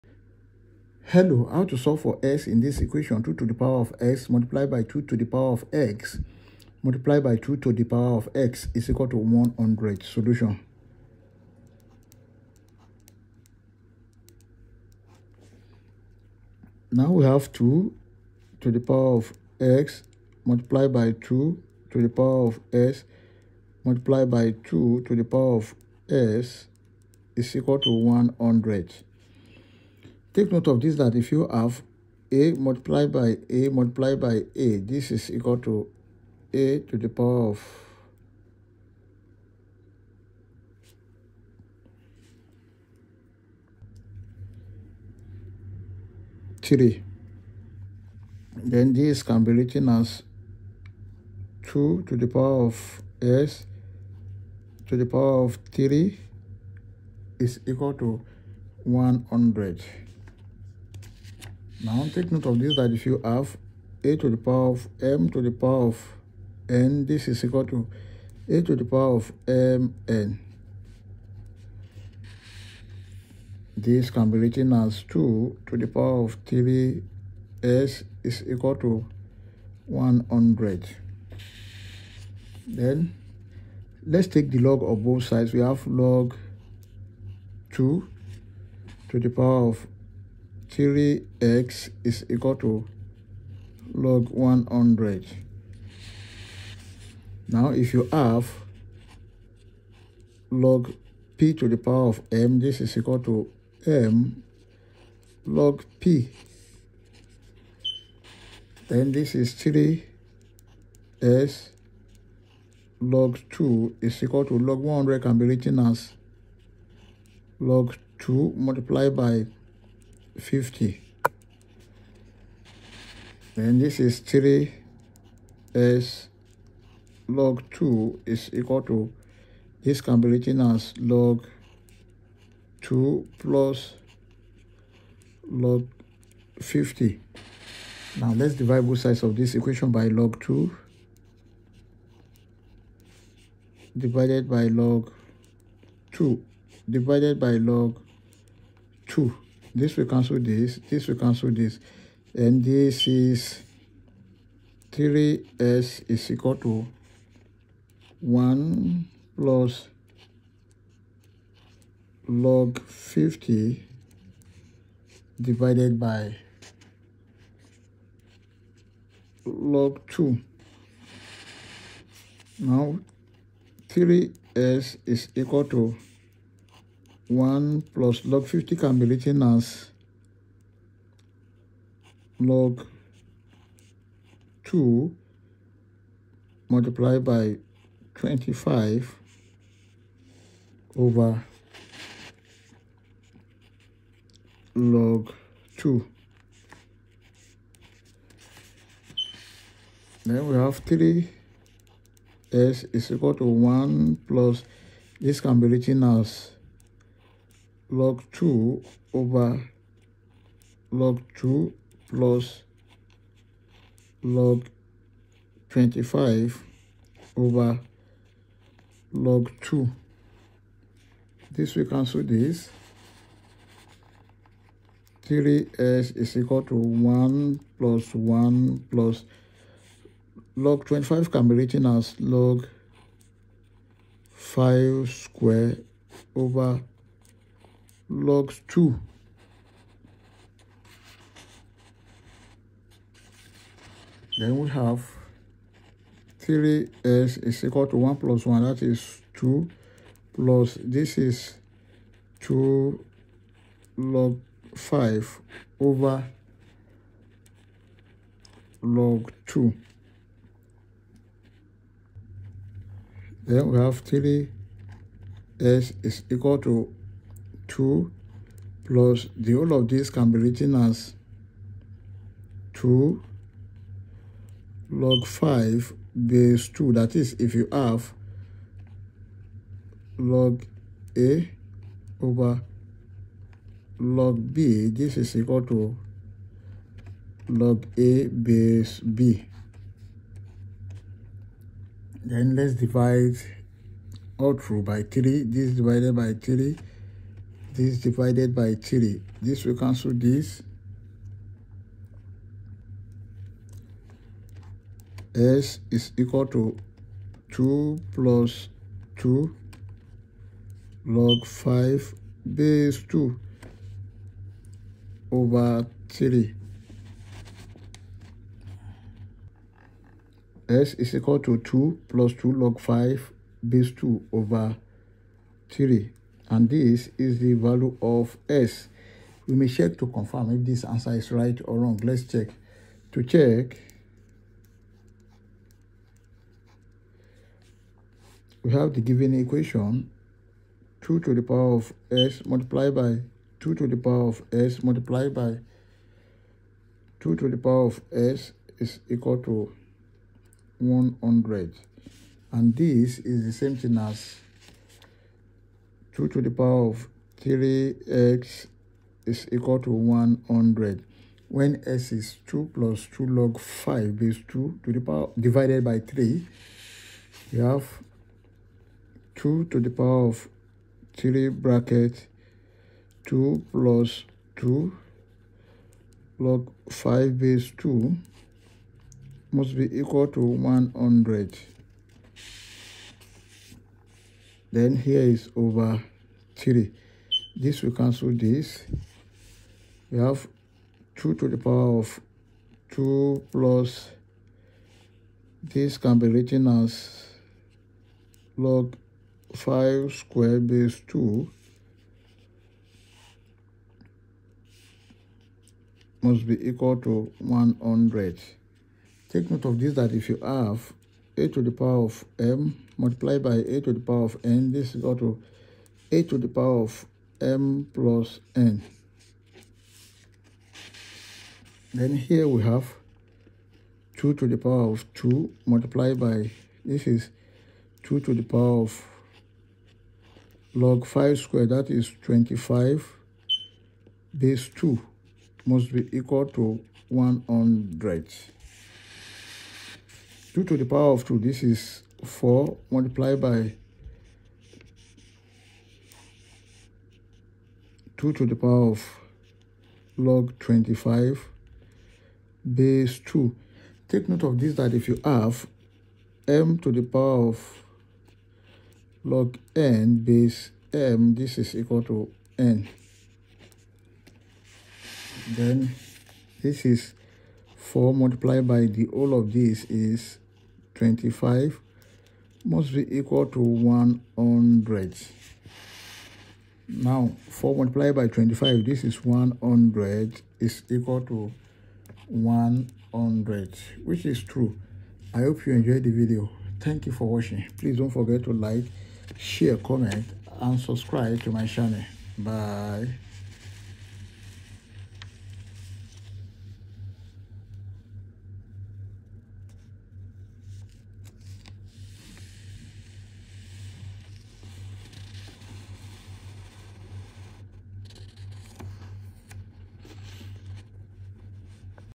Indian | Can You Solve This??? | A Nice Olympiad Algebra
In this Math Olympiad problem, we tackle the equation  and find the value of  with a clear, step-by-step solution. Perfect for math enthusiasts looking for a challenge!